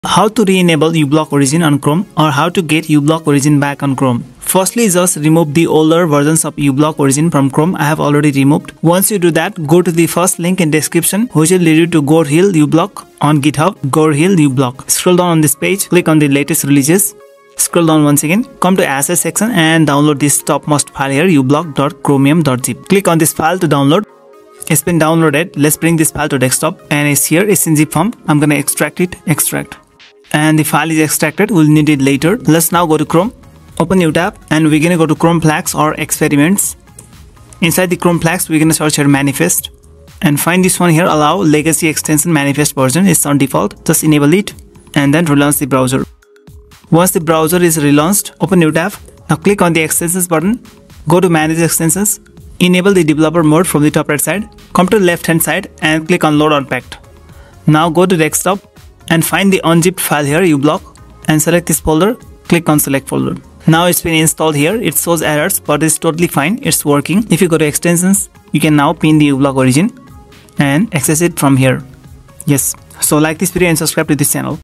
How to Re Enable UBlock Origin on Chrome | Install Ublock Origin in Chrome Again
This video guides you how you can re enable Ublock Origin extension on chrome. Install and use Ublock origin on Chrome again with this new trick.

2. Chrome Flags: chrome://flags/

Declaration: This video and this platform do not endorse any illicit undertakings. The materials presented on this channel are intended solely for educational, informative, or entertaining purposes.